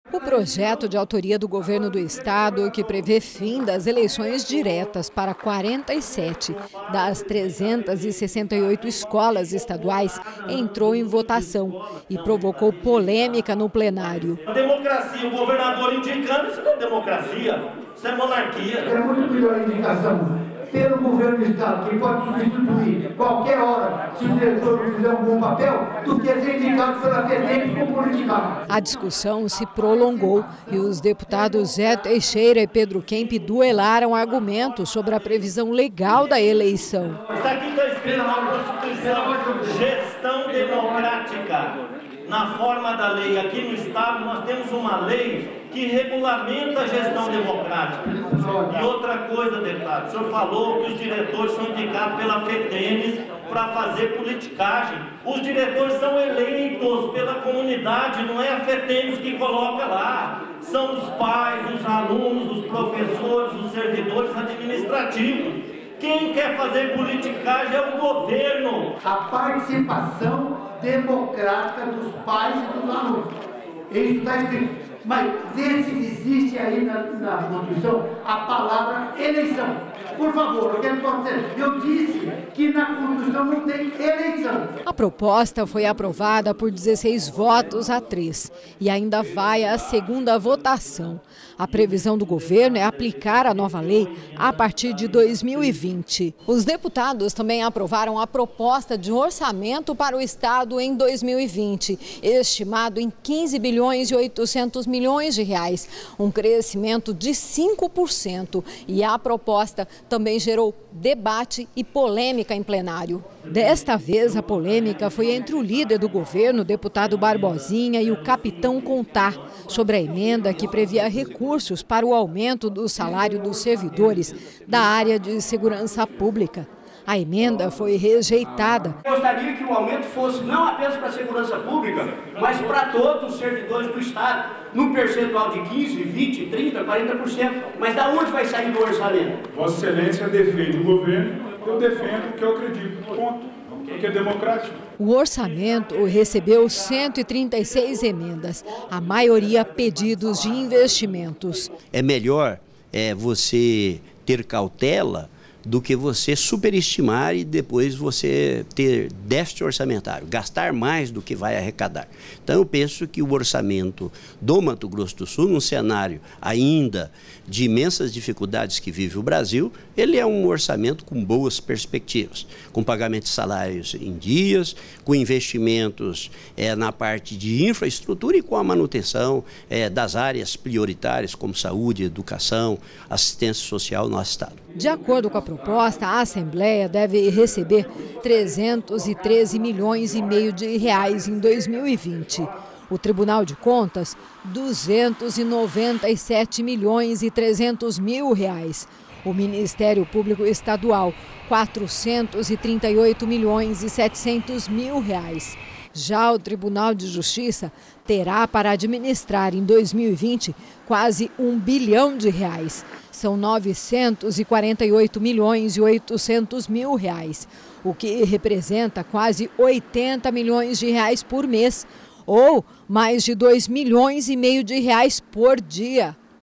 Fim das eleições diretas para diretores em escolas estaduais gera polêmica na Assembleia
A proposta de orçamento do Estado para o ano que vem, aprovada nessa quarta-feira (11) na Assembleia Legislativa, prevê crescimento de 5%. Poderes como o Tribunal de Justiça vão receber mais de R$2,5 milhões por dia. Entre os dez projetos de lei analisados na sessão desta quarta-feira (11), o que trata do processo de seleção dos dirigentes escolares na rede estadual foi o que mais provocou polêmica e debate entre os deputados.